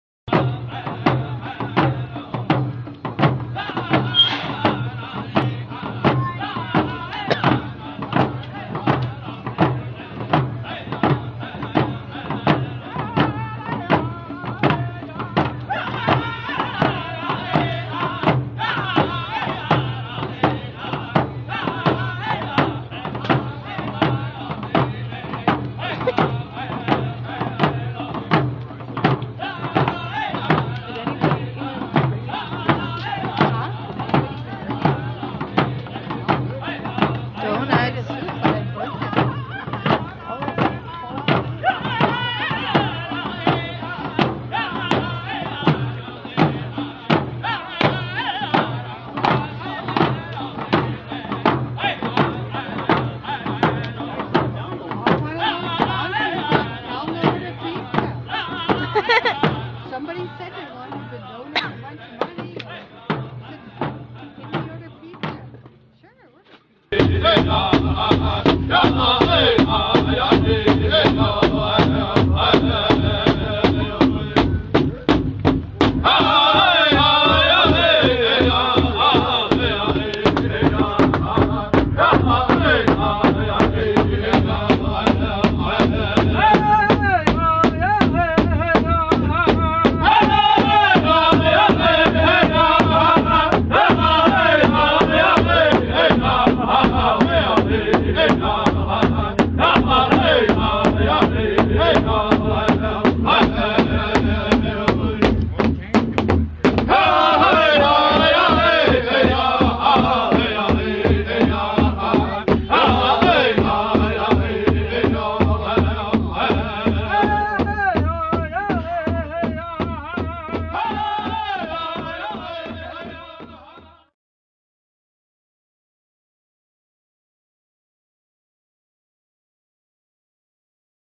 Aamjiwnaang Blockade - Drumming
I had the chance to go visit with the peeps at the blockade for a couple of days got some videos of the drumming that goes on there. Was good to see a Sarnia Police Officer stop by & drum as well! Idle No More!